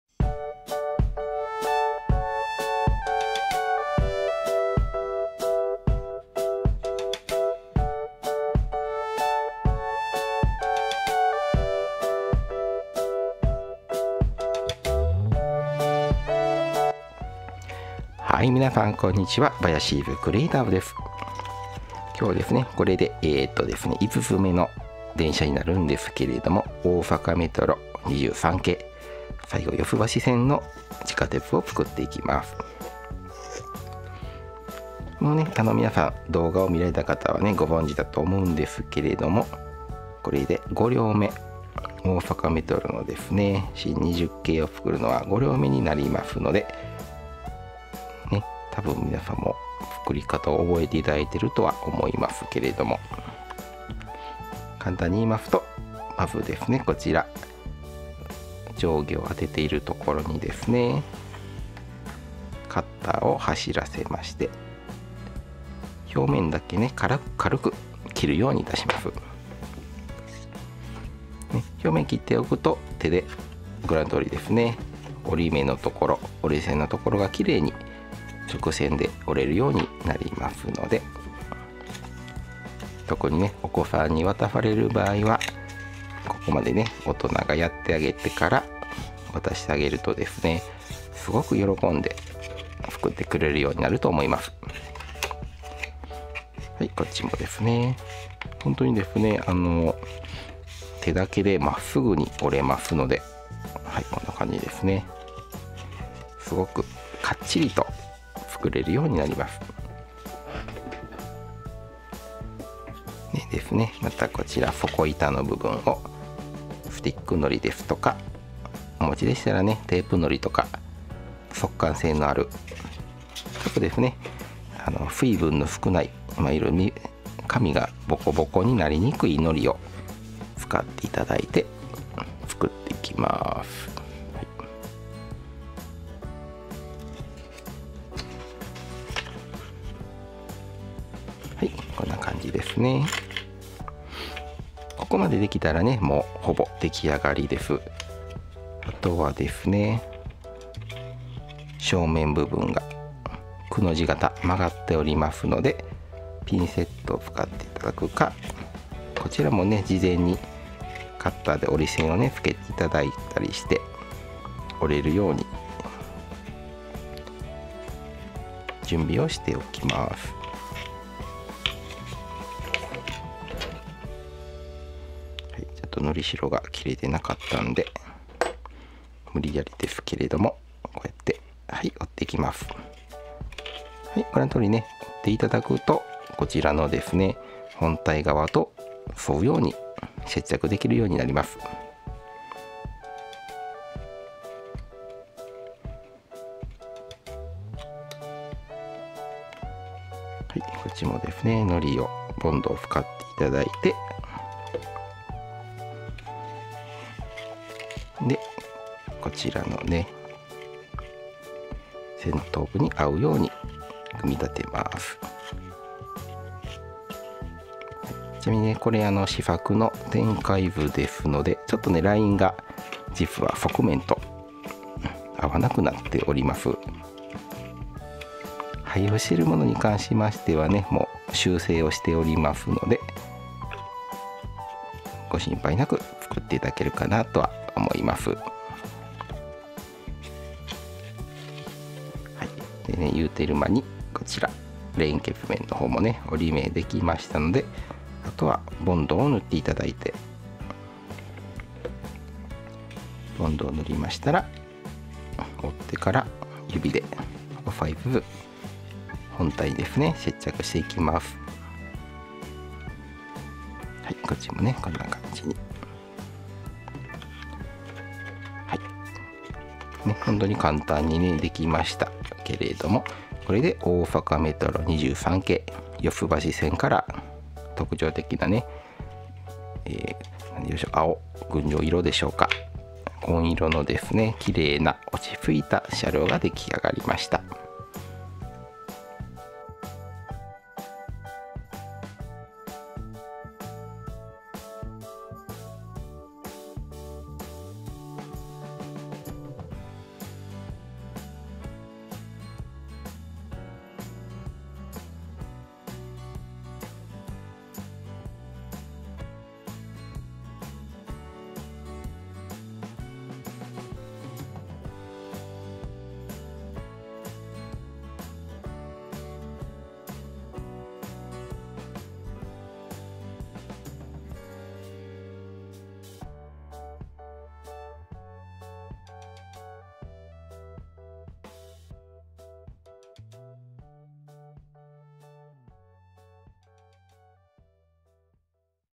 大阪メトロ 23系 四つ橋線のペーパークラフトを自作！
大阪ではおなじみの大阪地下鉄、現在は大阪メトロの主力車両を作ってみました。
御堂筋線と並走するような路線ですので、
動脈の御堂筋線と静脈の四つ橋線というような意味合いもあるとかないとか。